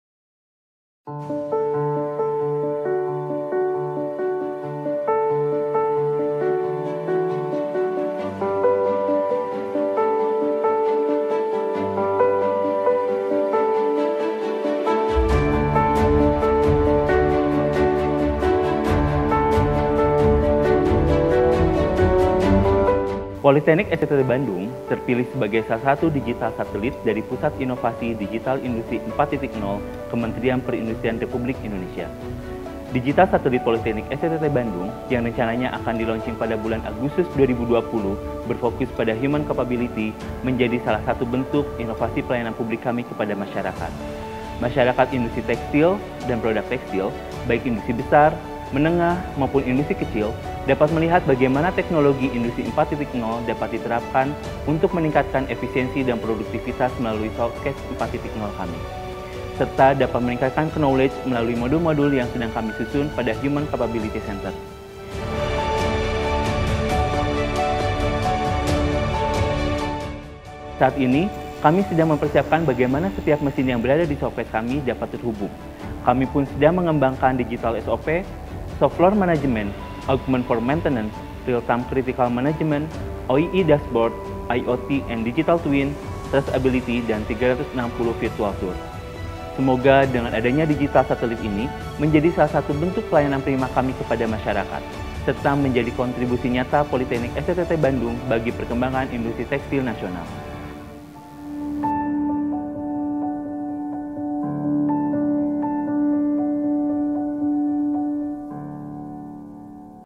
Preparation of Digital Satellite 4 0 Inovasi pelayanan Publik
Industri 4.0 memang sedang gencar-gencarnya di gaungkan di beberapa negara, Politeknik STTT Bandung sangat beruntung dapat mengimplementasikan industri 4.0 pada bidang tekstil dan apparel. Bagi masyarakat yang ingin tahu tentang Industri 4.0 di bidang tekstil dan apparel bisa banget nanti datang ke kampus Politeknik STTT Bandung.

KERENKHAAAN.